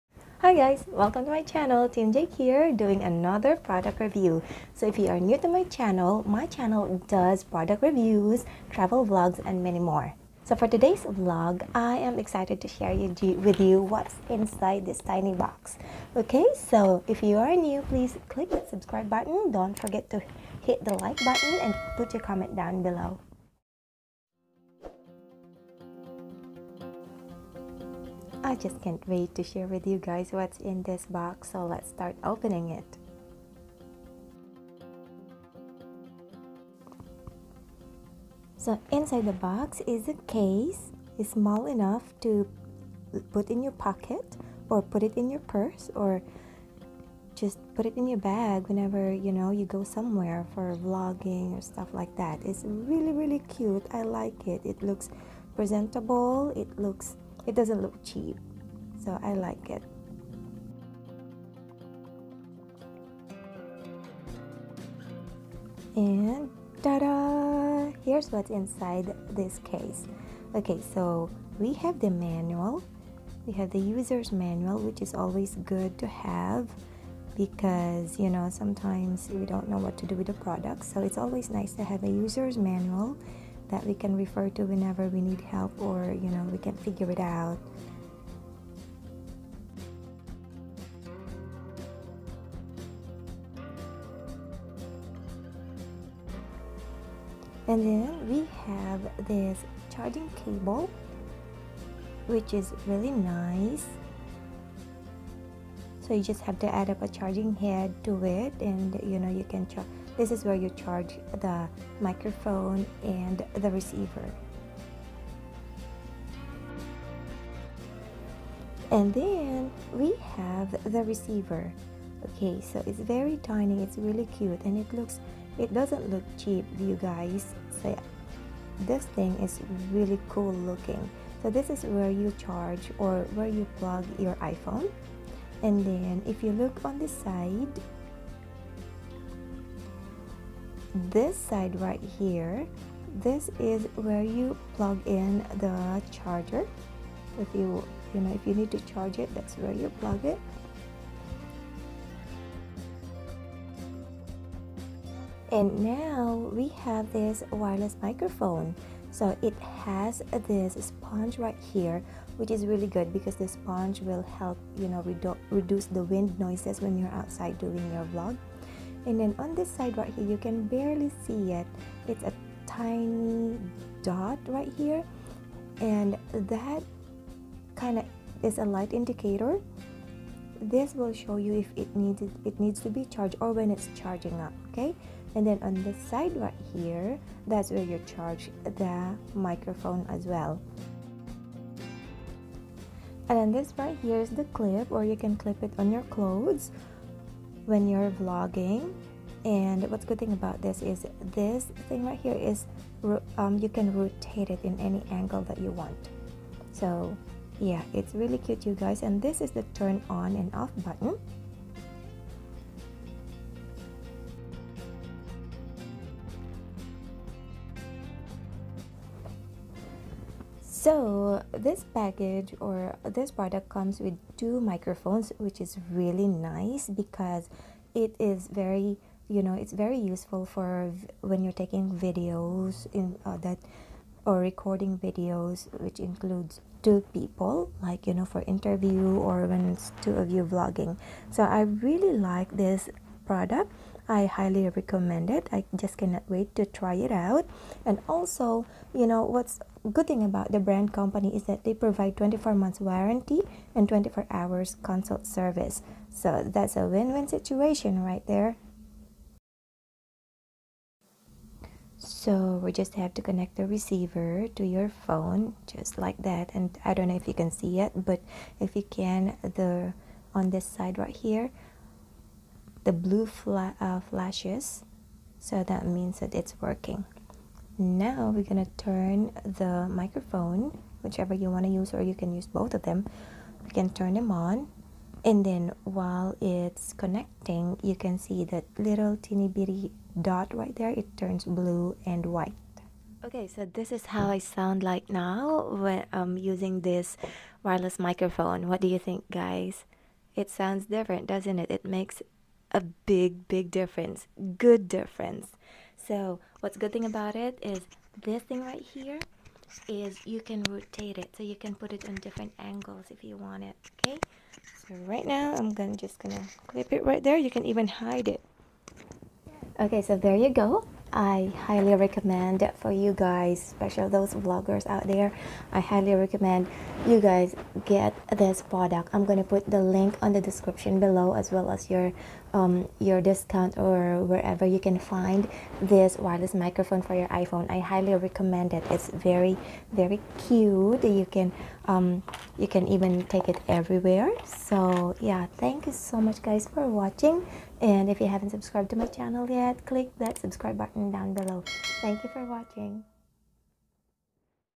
Best Dual Wireless Microphone for Iphone
This video content is a review on CKOKC Dual Wireless Microphone. You can check out the product on Amazon. Simply type in CKOKC, the store is called XSDUS, and the product model is K2.